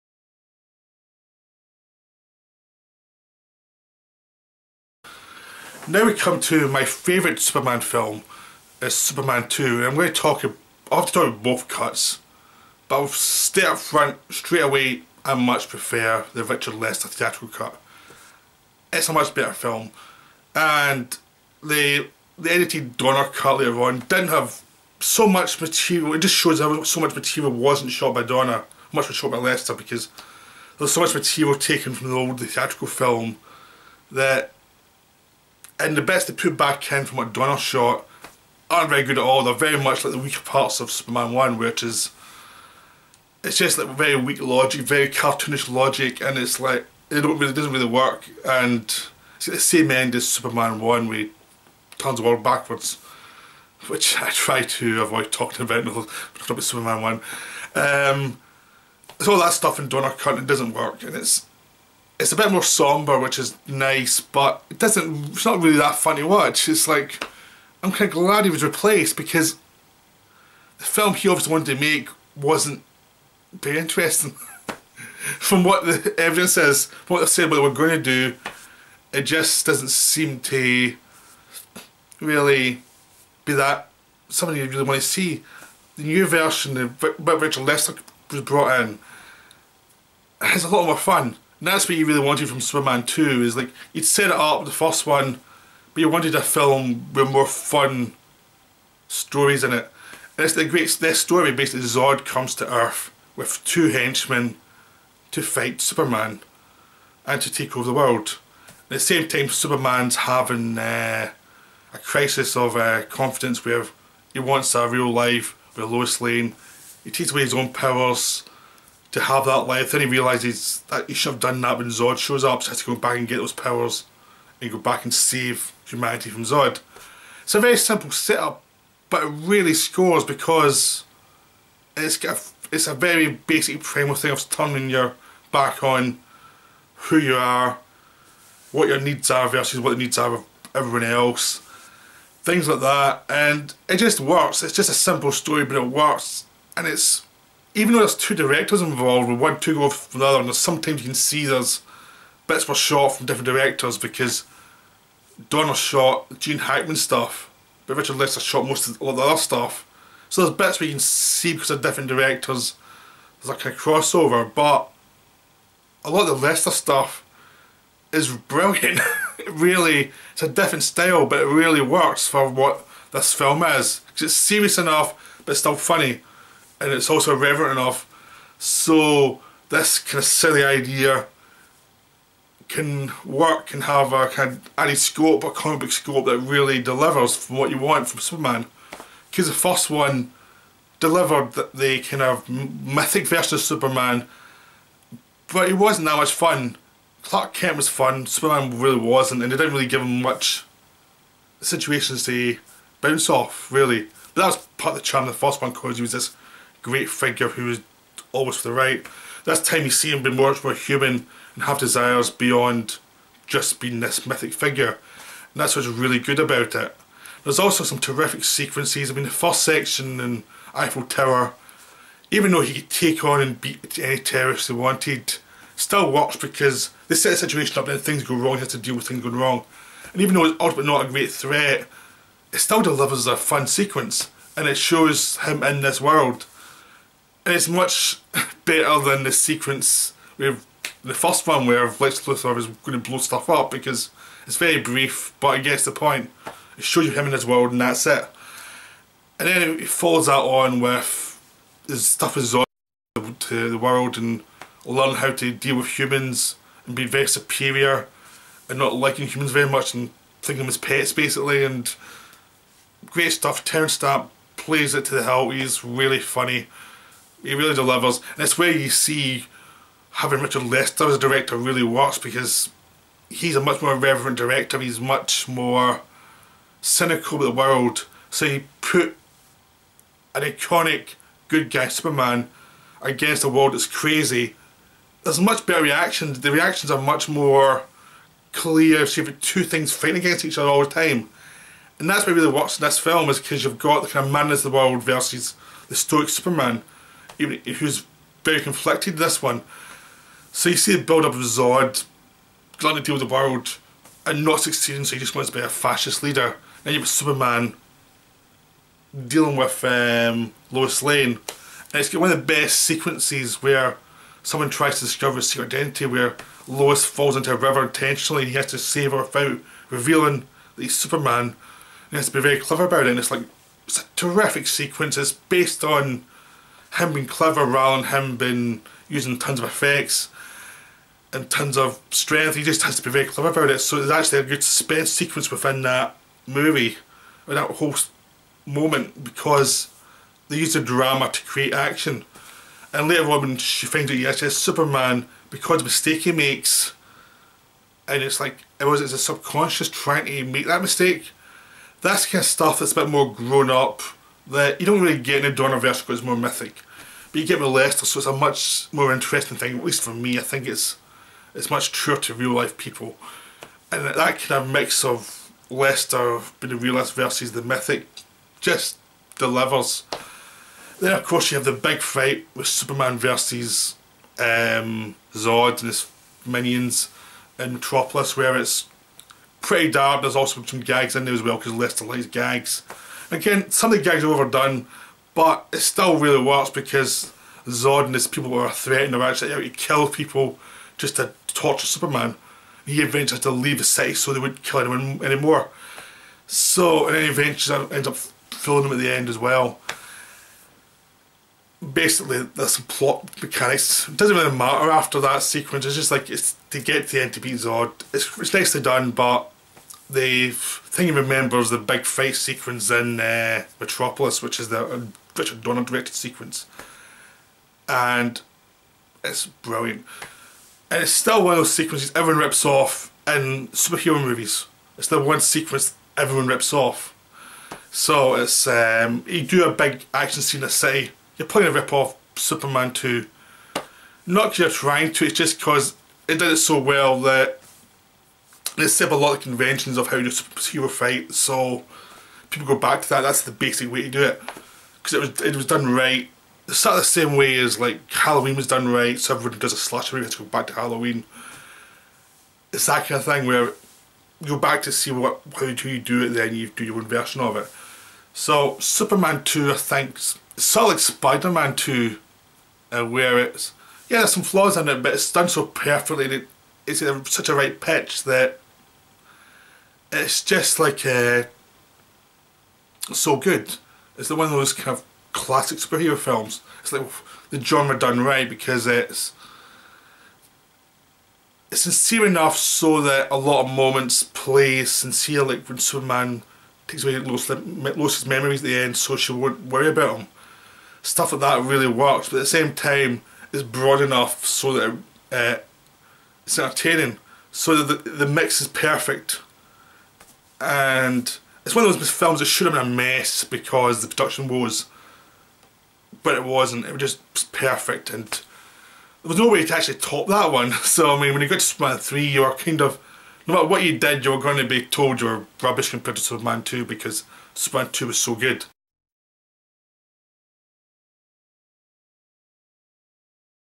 Superman 2 - The Damned
The Damned looks at Superman 2, the greatest superman film ever, where Superman faces the amazing General Zod.